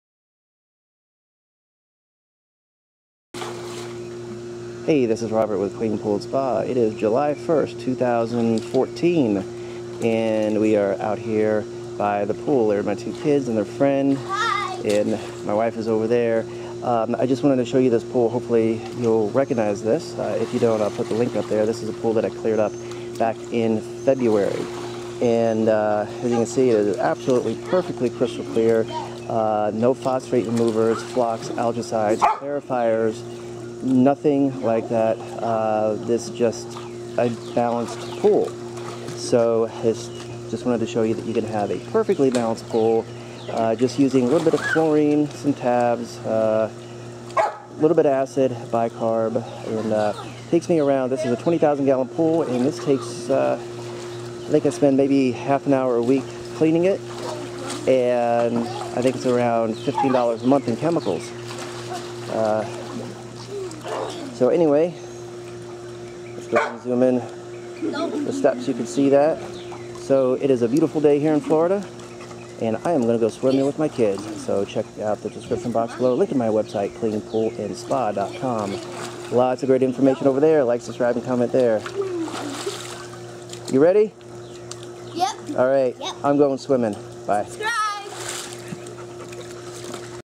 Crystal Clear & Perfectly Balanced Swimming Pool Water
for more swimming pool care maintenance tips and instructions about how to have a crystal clear and perfectly balanced swimming pool.

This is a 20,000 gallon pool I cleared up back in February 2014. It takes about 20 - 30 minutes to clean each week with a cost of about $15 in chemicals per month. I have not added one drop of phosphate remover, algaecide, clarifier, coagulant, flocc, or any "extras" in the last four months that "they" say is required to have a perfectly balanced and clear pool.

The pump runs 12 hours a day and I clean the cartridge filter once per week.

The chemical readings that day were:

CL 2.8ppm
pH 7.6
TA 110ppm
CH 280ppm
CYA 40ppm

This pool has only been shocked once in the last 4 months. We had heavy rains and the chlorine dropped to 0ppm. It got a little cloudy so I shocked the pool with 2 gallons of liquid chlorine.